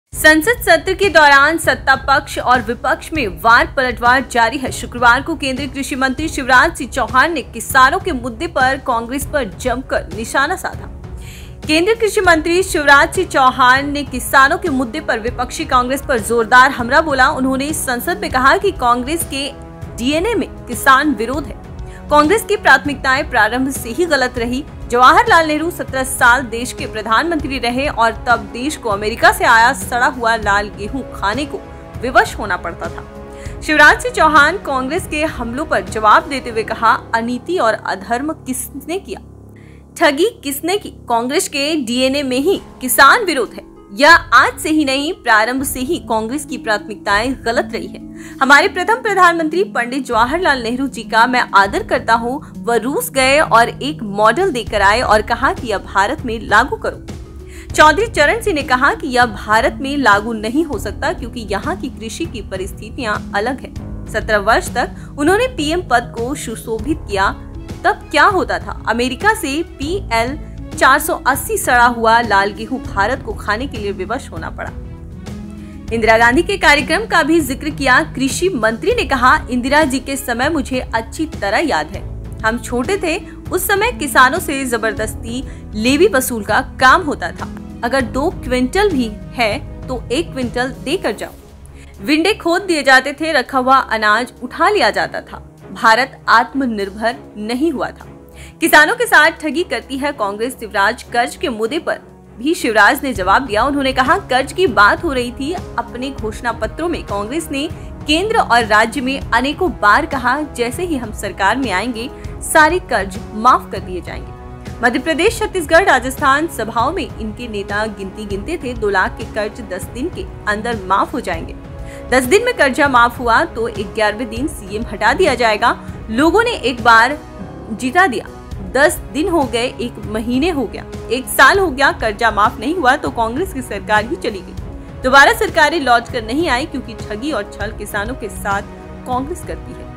Shivraj Singh Chauhan का Congress पर हमला: 'जब Nehru PM थे, देश को सड़ा हुआ गेहूं खाने को...'
Parliament Session: संसद सत्र के दौरान सत्तापक्ष और विपक्ष में वार-पलटवार जारी है। शुक्रवार को केंद्रीय कृषि मंत्री शिवराज सिंह चौहान ने किसानों के मुद्दे पर कांग्रेस पर जमकर निशाना साधा।